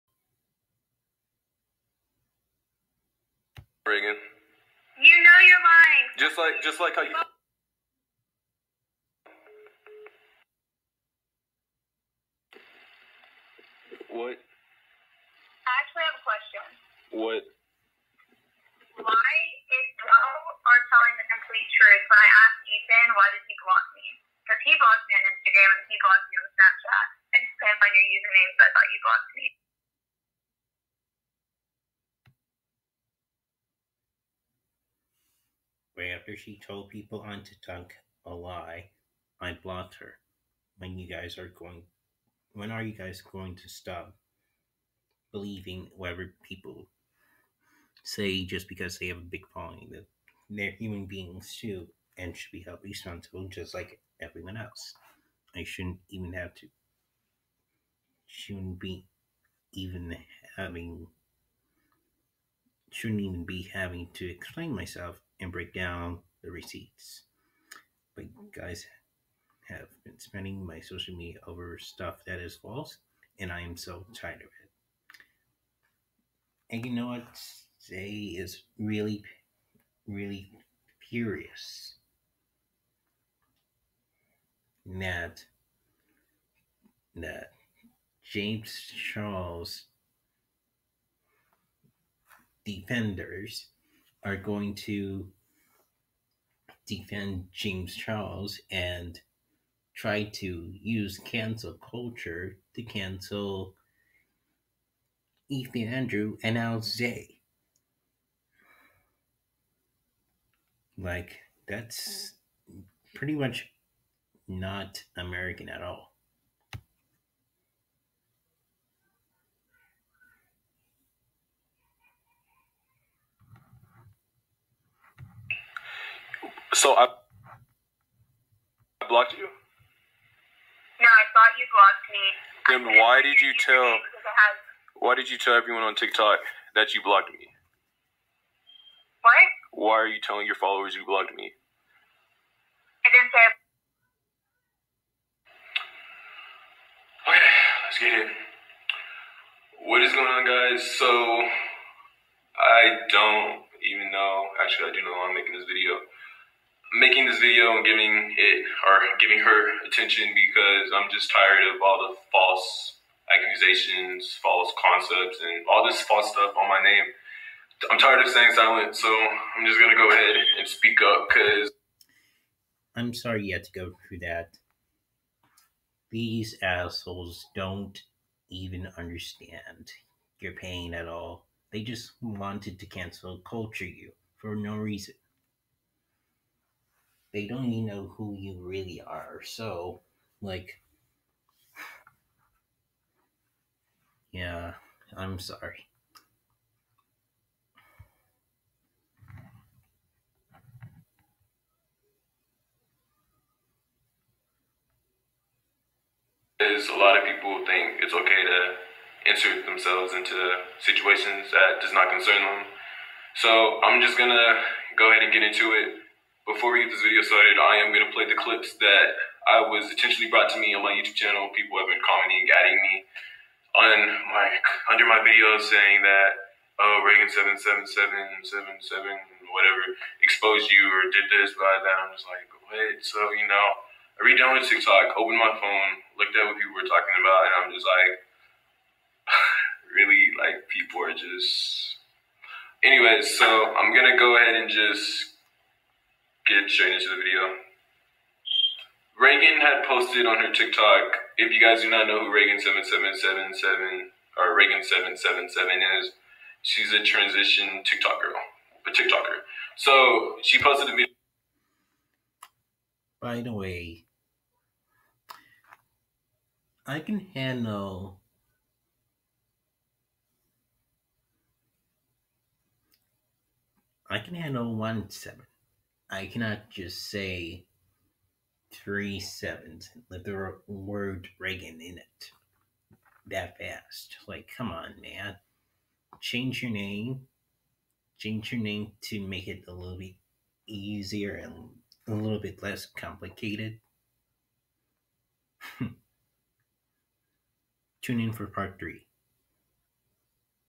Zay Fuego DESTROYS Rhegan777! Part 2 |RTWRP #82
Zay Fuego DESTROYS Rhegan777! Part 2|

Zay Fuego told his truth about Rhegan Coursey (Rhegan777) after she claimed she knew how old Ethan Andrew Rhett and Zay Fuego was but Zay confirmed that he ONLY met her once and never hung out and talked all the time. Rhegan lied saying she knew Ethan's age saying he's 18, but Ethan Andrew is actually 14. She also claimed she knew them 2-3 years ago but she also lied. Rhegan however understands that James Charles shouldn't even replied in the first place.